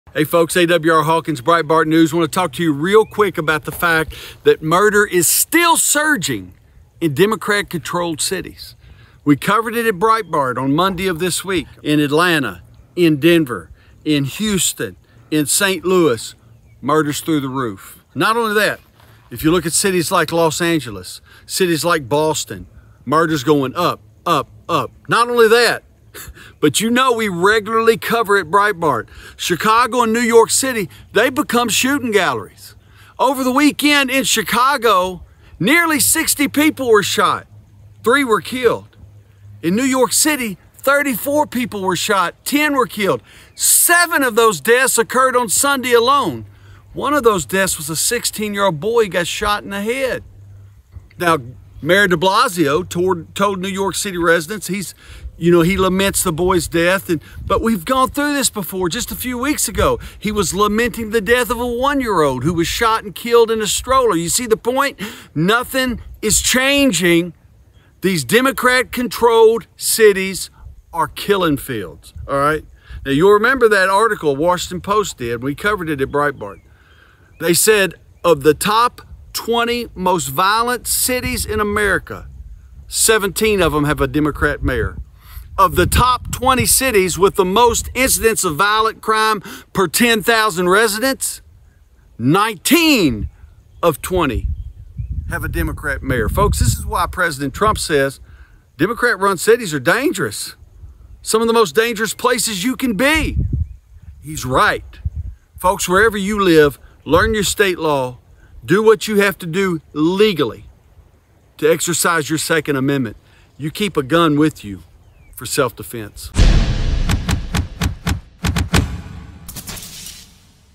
Hawkins: 'Nothing Is Changing' — 'Democrat-Controlled Cities Are Killing Fields'
Breitbart News's AWR Hawkins reiterates how deadly Democrat-run cities have become due to their strict gun laws. Visit Breitbart.com/Downrange to equip yourself with the latest statistics and news articles to defend your right to bear arms.